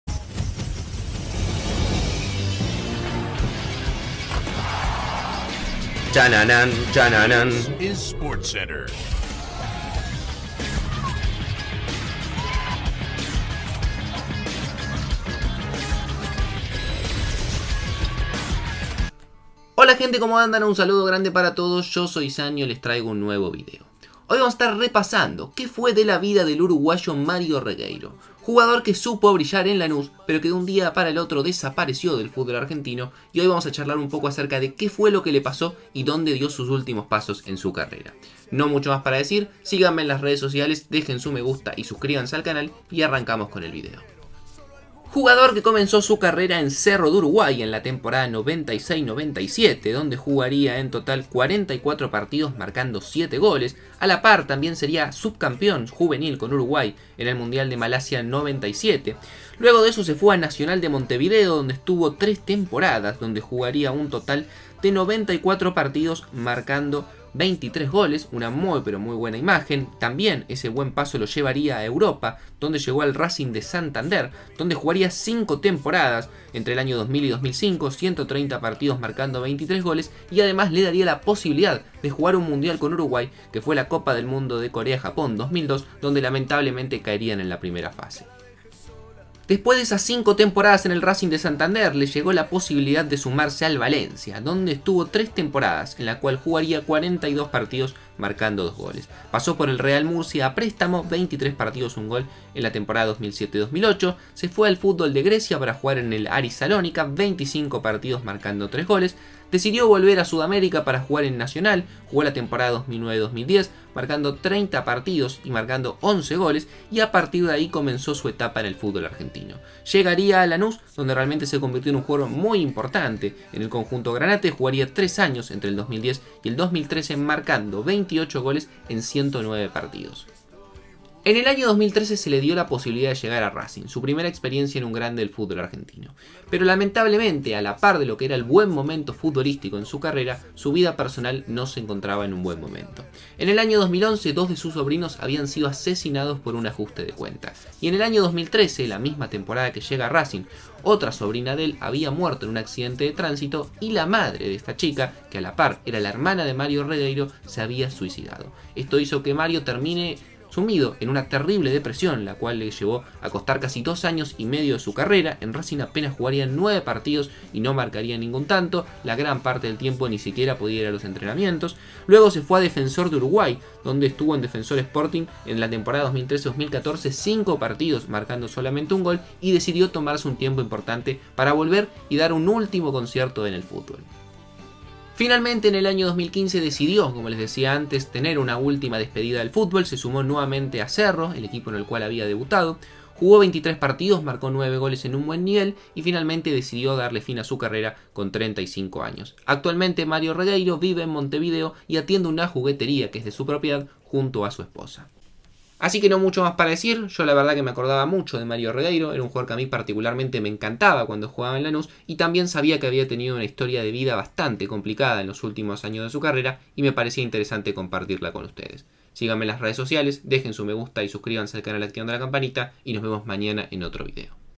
¿Qué fue de la vida de Mario Regueiro?
Como andan? Hoy vamos a repasar la carrera del uruguayo que supo brillar en Lanus, y los problemas que lo alejaron del futbol.
Abrazo James.